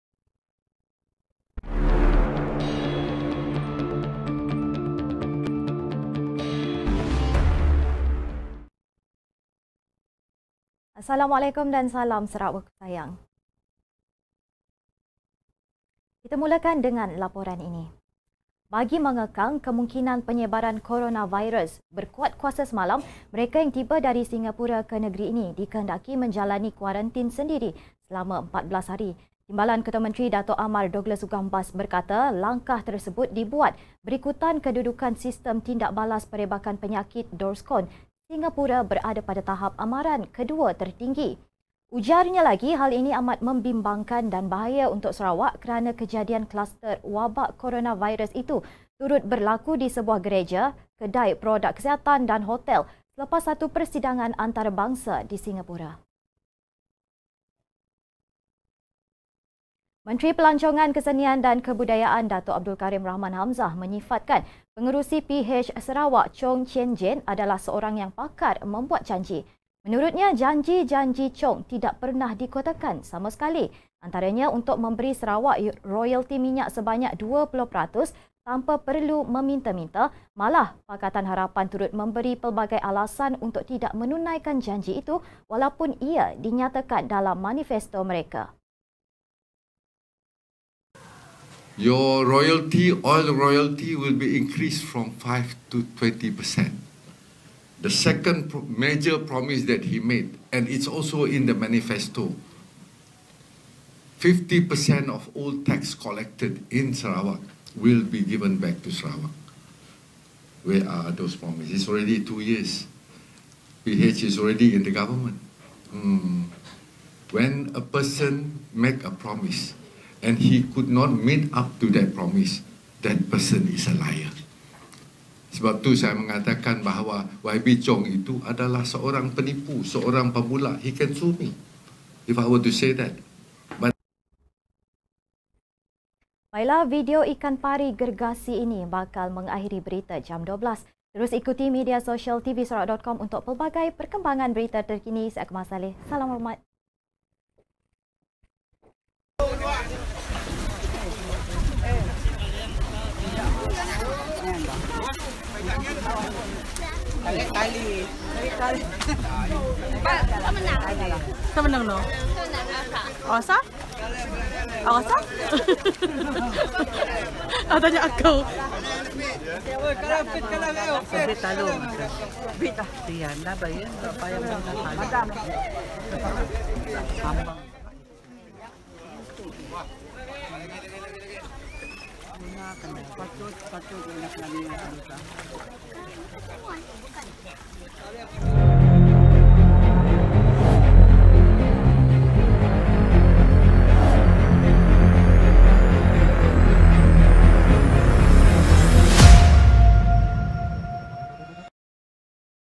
KORONAVIRUS: Sarawak Kuat Kuasa Kuarantin Untuk Pelancong Singapura
1. KORONAVIRUS: Sarawak Kuat Kuasa Kuarantin Untuk Pelancong Singapura

2. Chong Seorang Pakar Membuat Janji - Karim